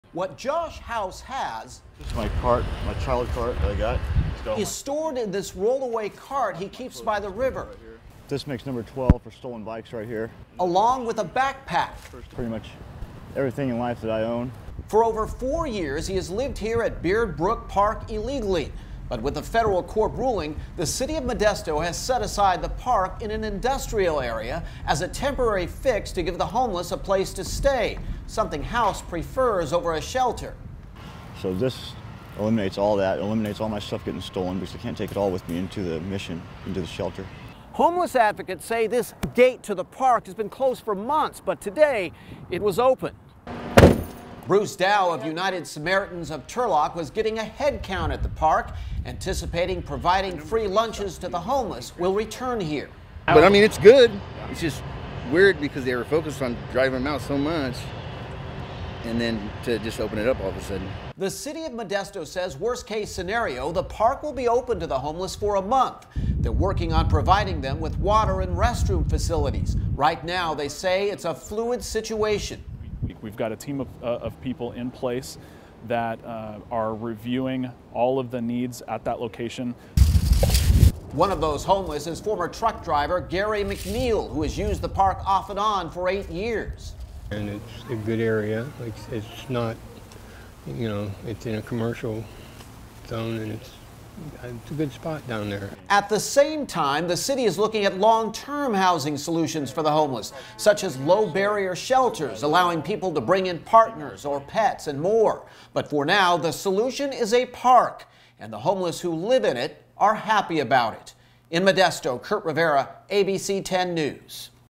Modesto implementing temporary solution to help homeless
The City of Modesto is implementing a temporary solution to help the homeless after a new court ruling now prevents park rangers from issuing illegal camping citations.